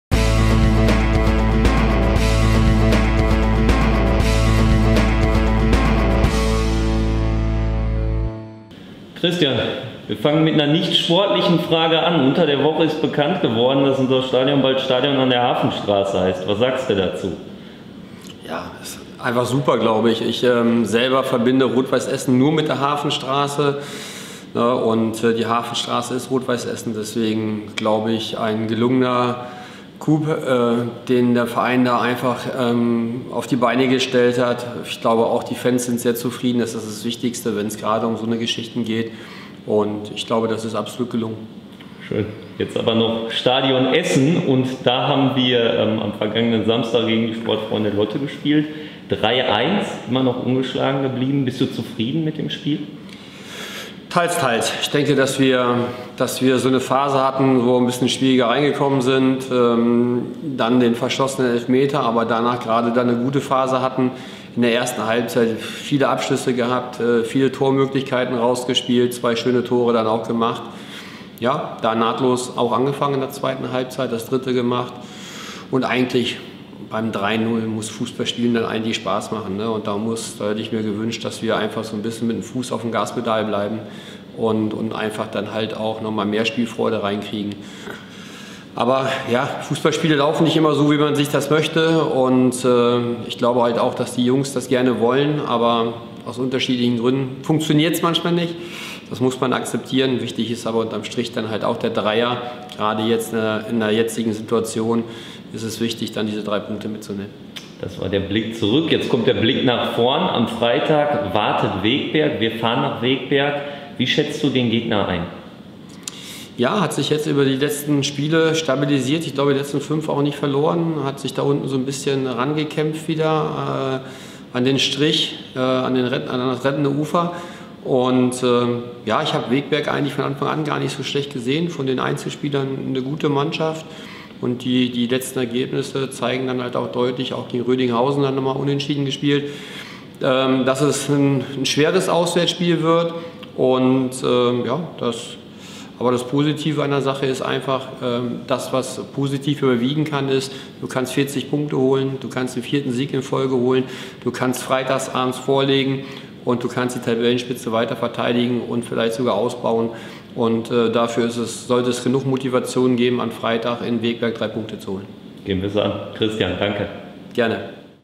Christian Neidhart vor dem Wegberg-Spiel: "Vierten Sieg in Folge holen!"
Christian Neidhart spricht im Videointerview vor dem Duell gegen den FC Wegberg-Beeck über den neuen Stadionnamen "Stadion an der Hafenstraße",  das vergangene Lotte-Spiel und die anstehende Aufgabe.